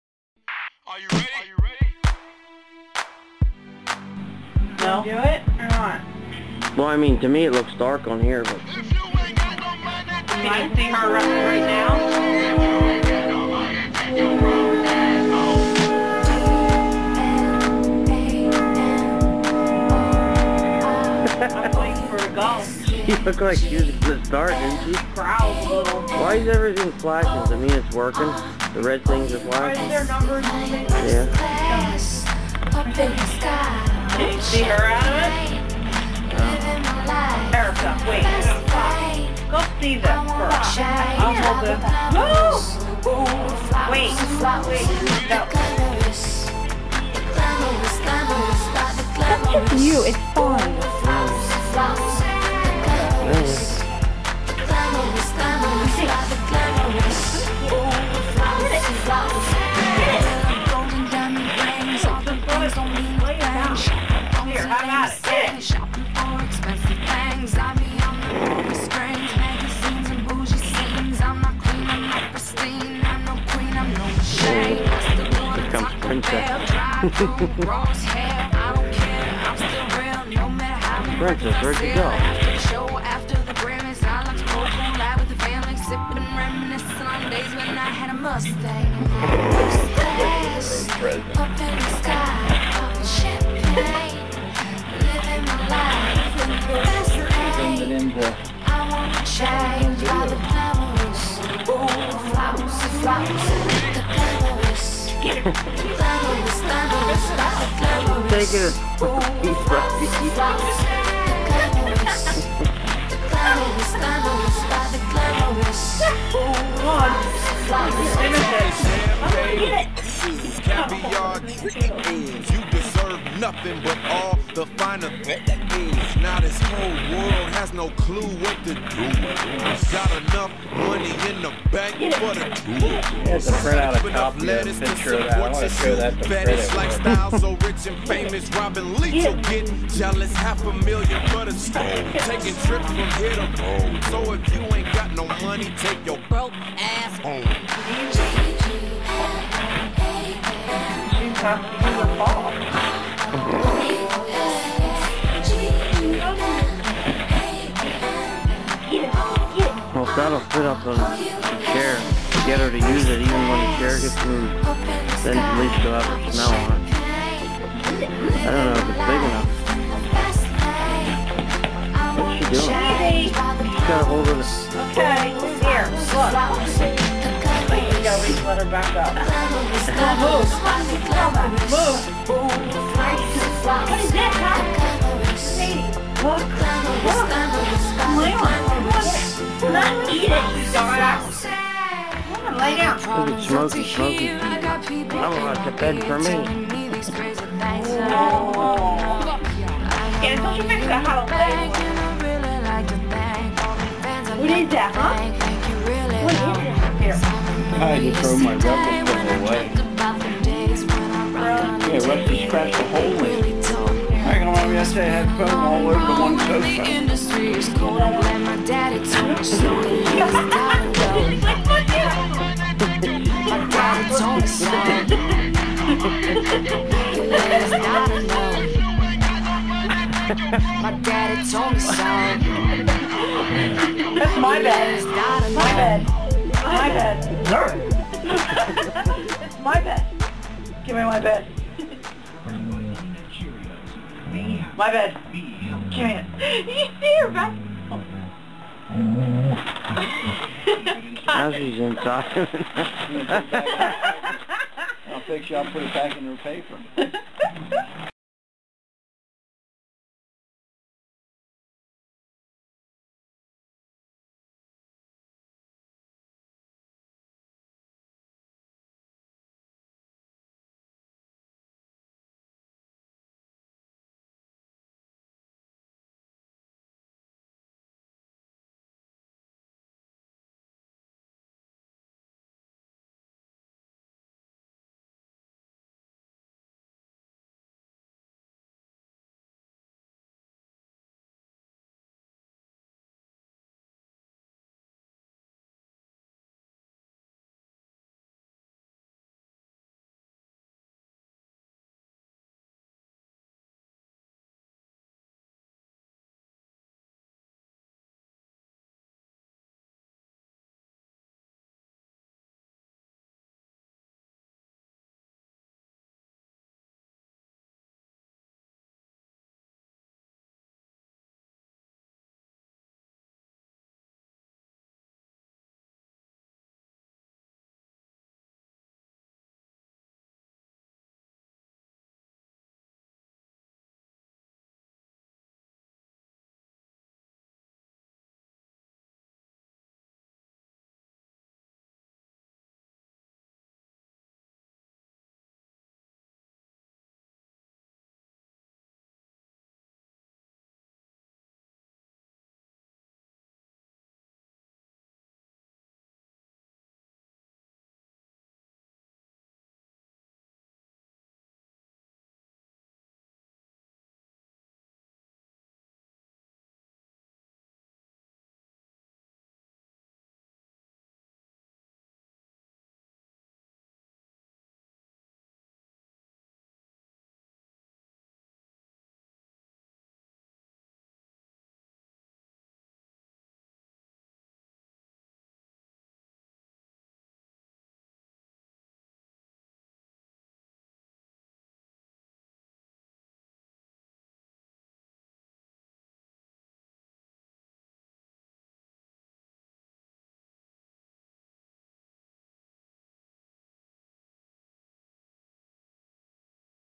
Our Rottweiler Mercedes' 10th Christmas
Our Old Girl Mercedes Opening Her Christmas Present One Piece At A Time.